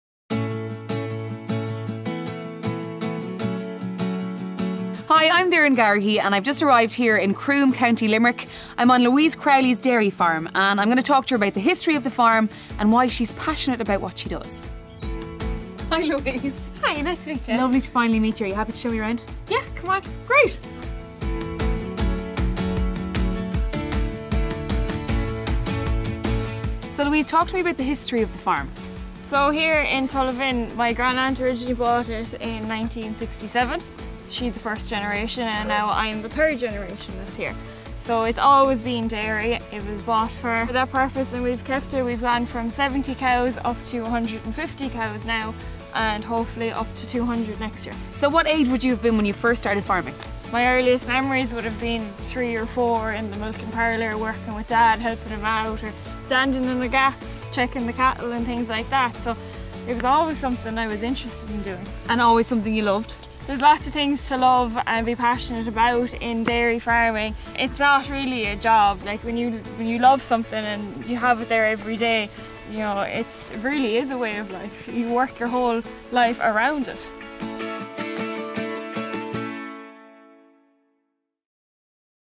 Generation to Generation, Family Farming | Doireann Garrihy visits Irish Dairy Farm
This video tells a story of generation to generation farming through the Crowley Farm in Co.Limerick. Doireann Garrihy, Radio Broadcaster & Social Media Personality visits Limerick dairy farm to learn more about the history of the farm and the partnership between Louise Crowely and her father and her future plans for the farm.